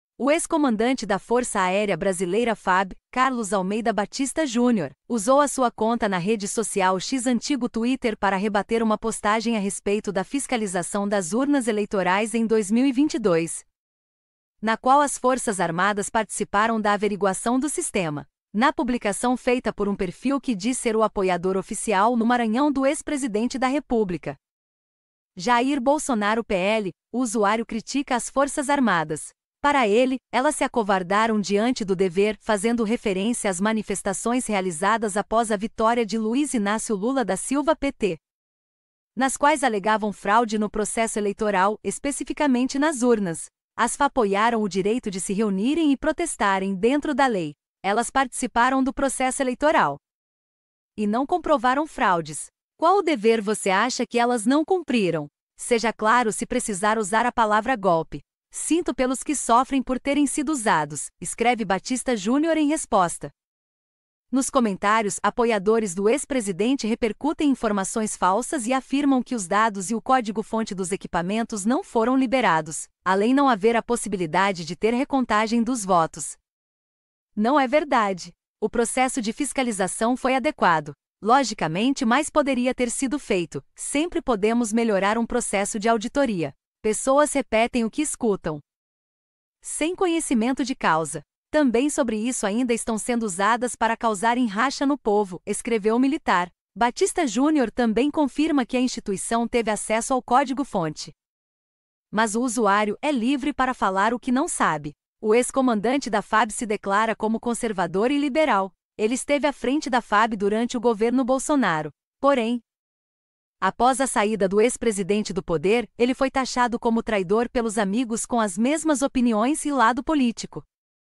Ex-chefe da Aeronáutica defende a fiscalização feita nas urnas eletrônicas em 2022. ‘Foi adequado’
😆😂 Obrigado novamente!😂😆
💗📌 Brazil